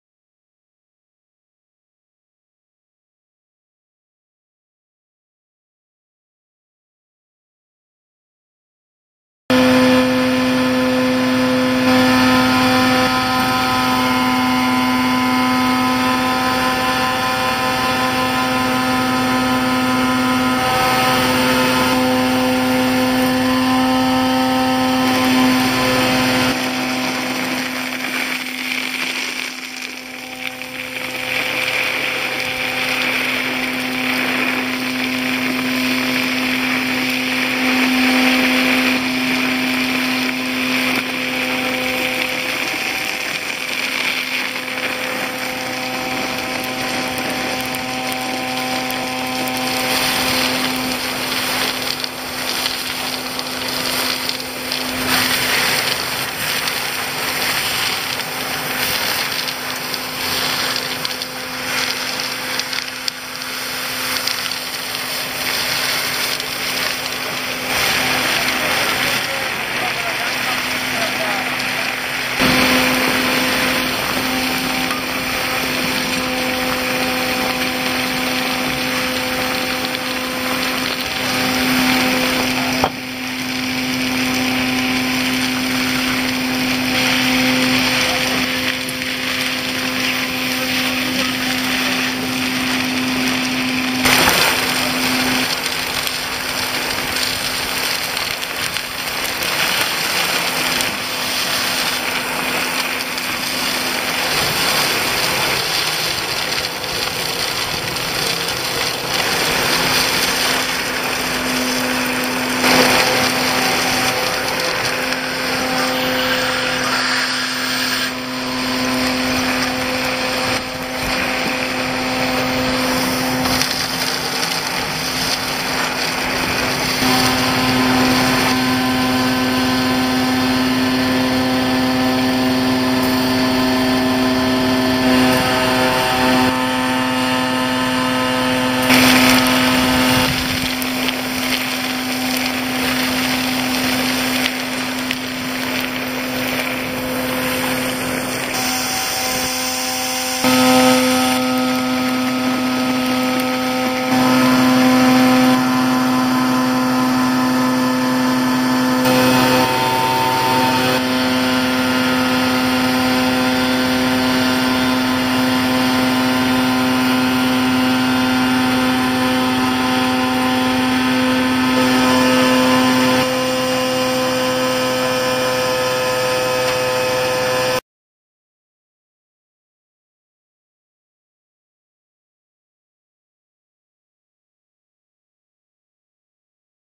CAPBORA - Asp. ghiaia tetto di una banca
Camion CAPBORA, aspirazione della ghiaia da un tetto di una banca